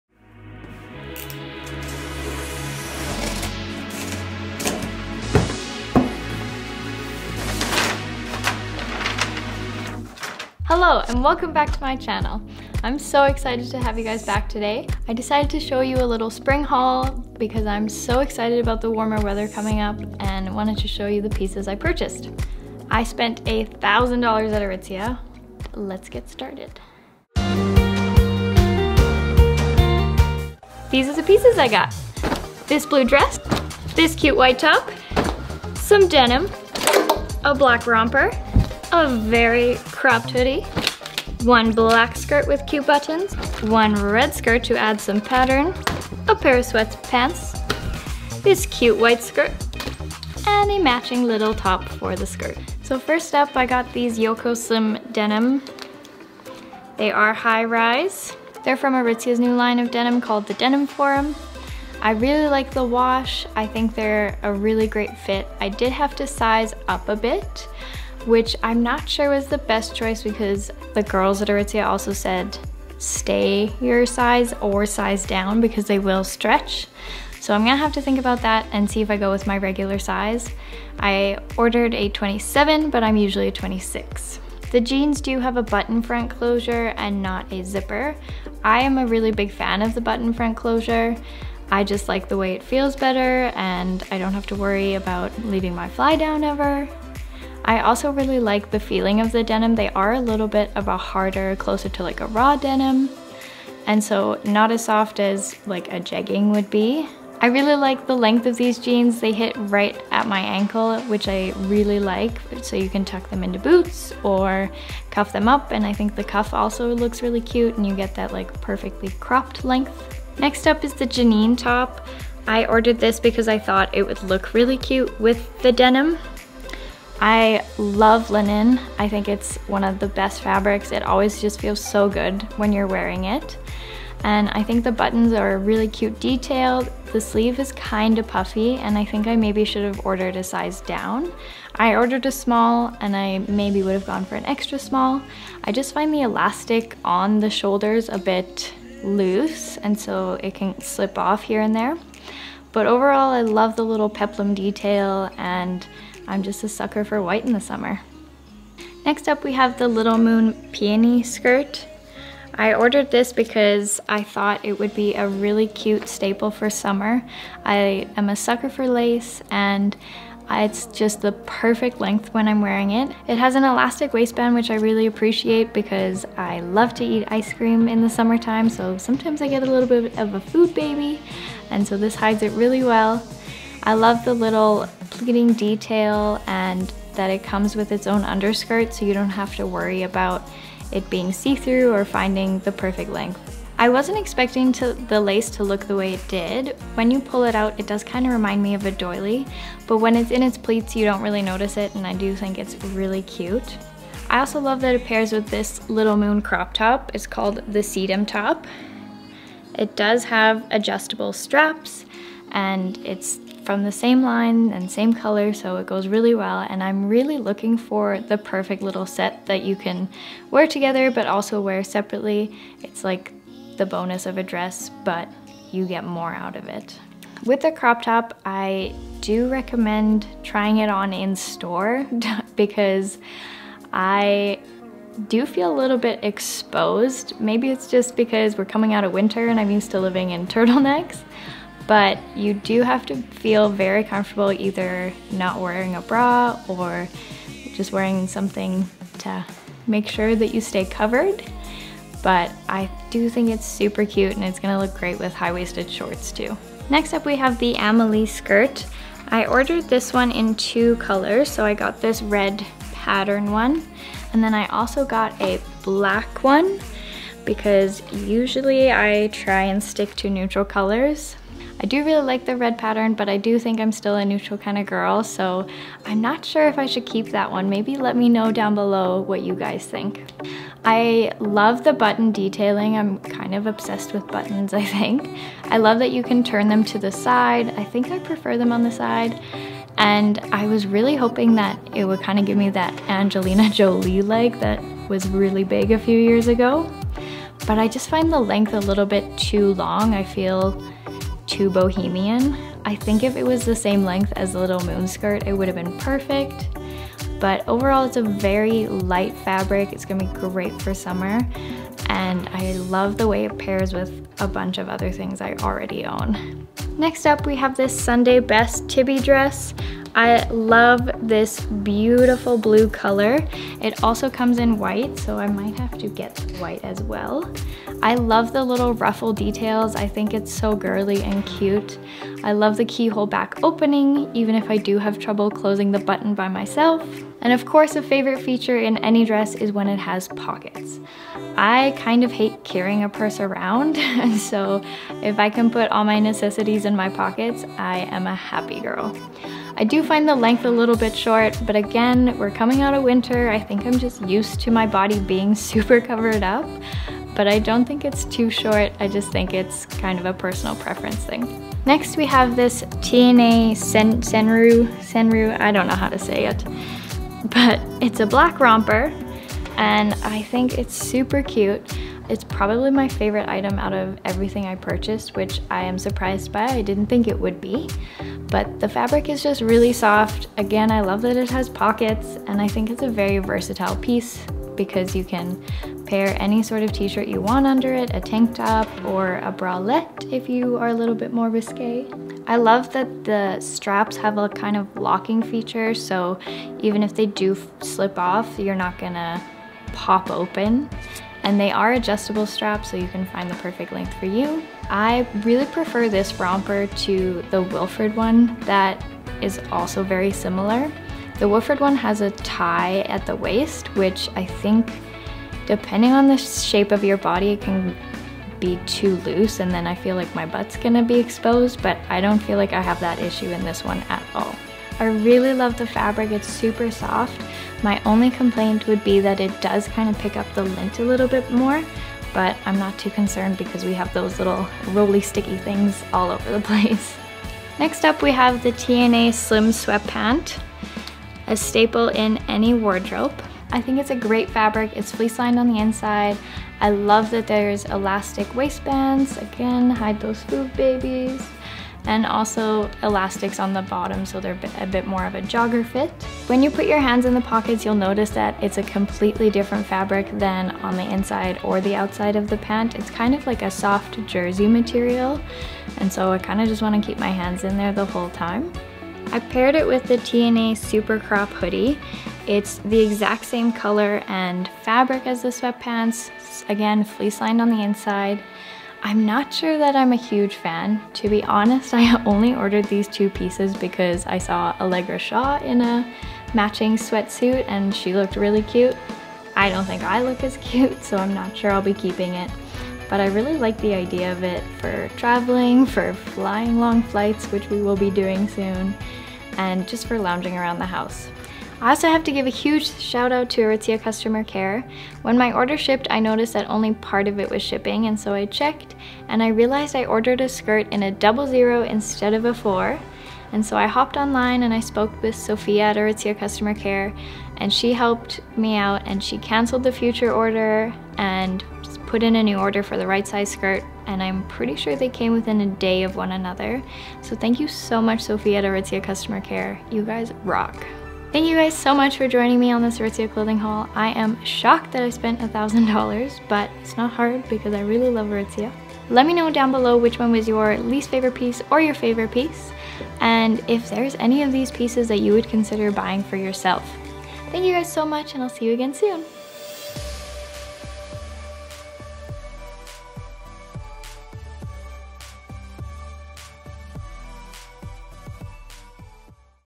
$1000 ARITZIA HAUL | 2019
Hey friends! Today I am sharing with you a clothing haul from one of my favourite clothing stores of all time! ARITZIA! If you follow me on instagram you would've noticed that the majority of my pieces are from there so I am very excited to be sharing some of my favourite pieces from their spring launch. I've include the links for each item below!

GEAR:
Sony A7III
Sony rx100 V

I hope that this video gave you some ideas of what spring and summer trends are happening in 2019 and what us Canadians in Calgary will be wearing in the coming months.